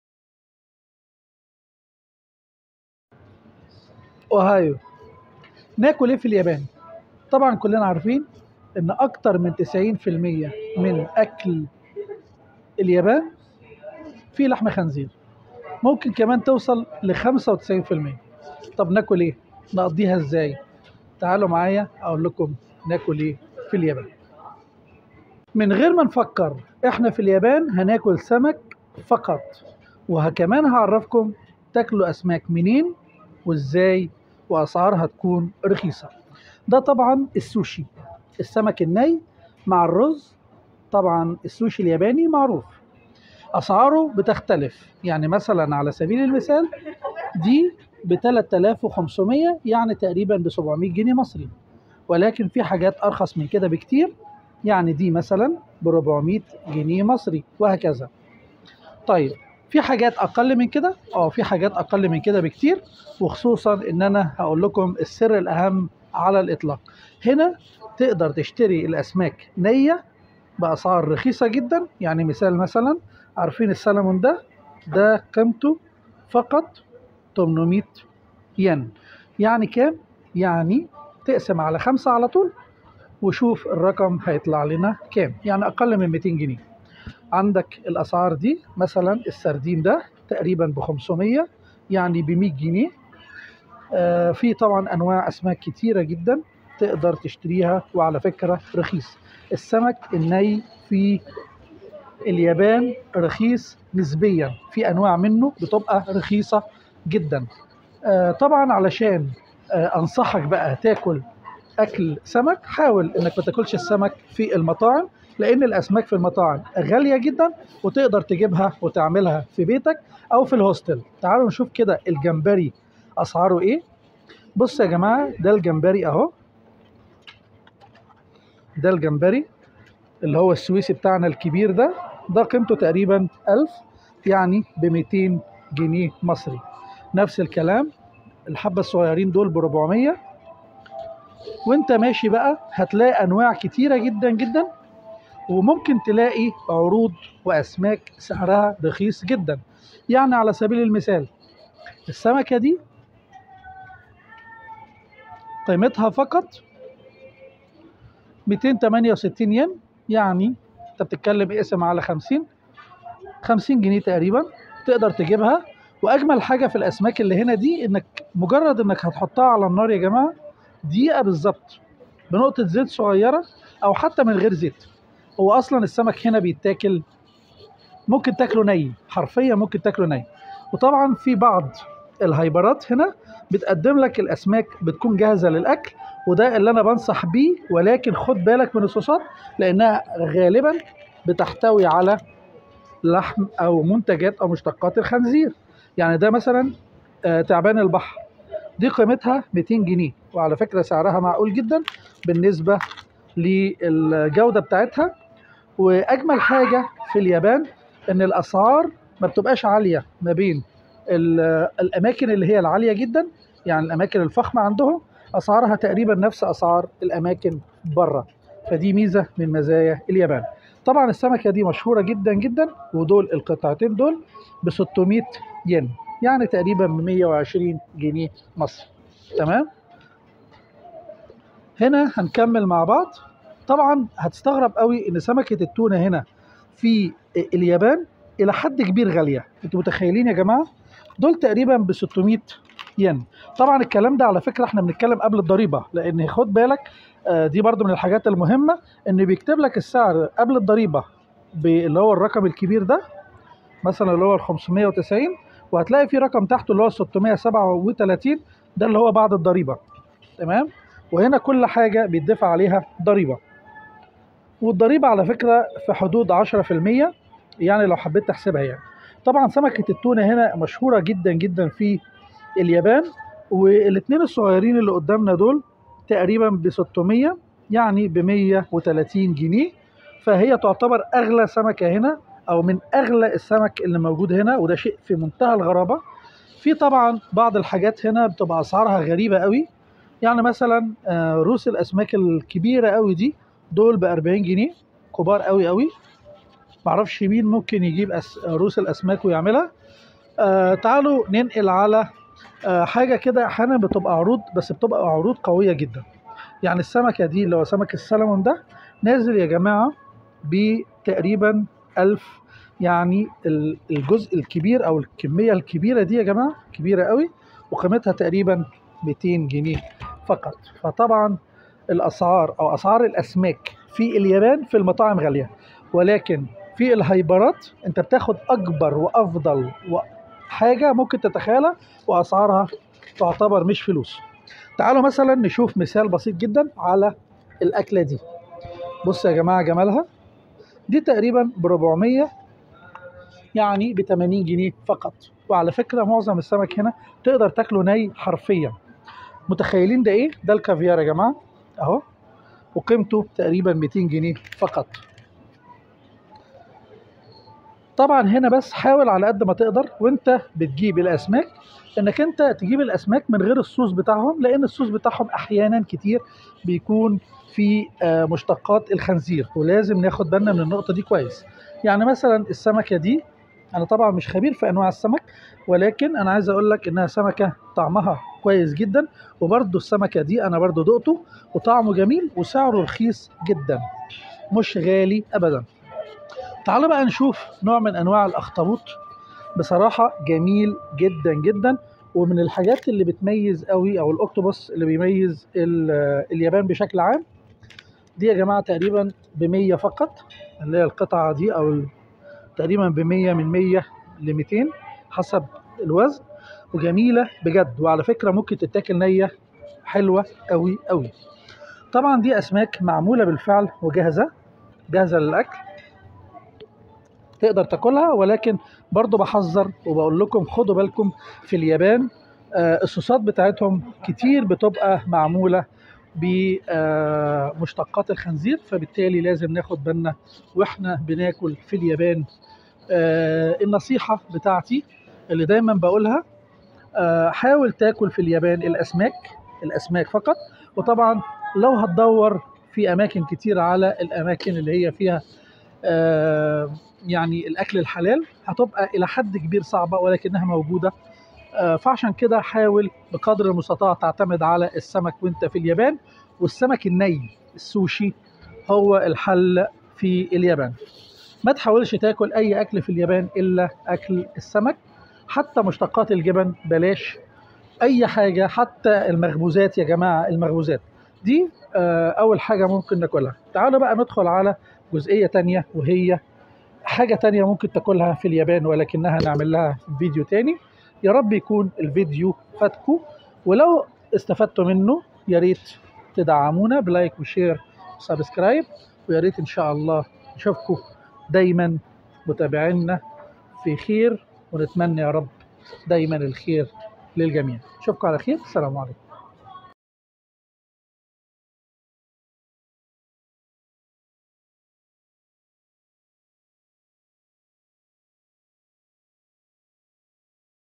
لو مسافر اليابان تعالي اقولك تاكل ايه في اليابان حلال ؟ وباسعار معقولة ؟ اماكن الاكل الحلال
الاكل _الحلال _في اليابان#سعر الاكل باليابان وطوكيو